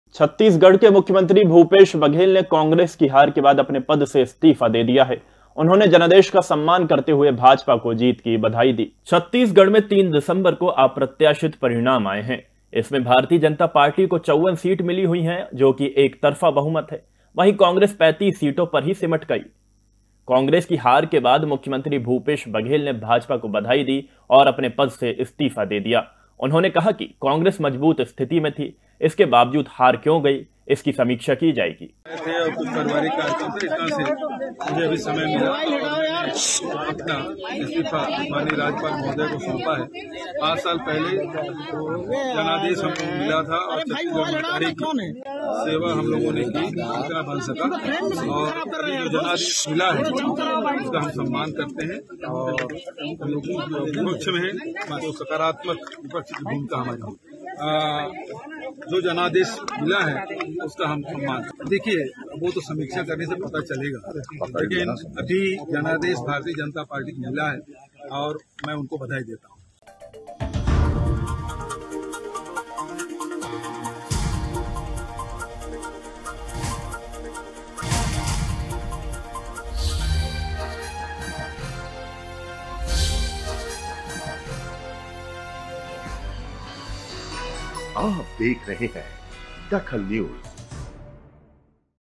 DAKHAL NEWS चुनाव हारने के बाद भूपेश बघेल का इस्तीफा
चुनाव हारने के बाद भूपेश बघेल का इस्तीफा
जनादेश का सम्मान,हार की समीक्षा की जाएगी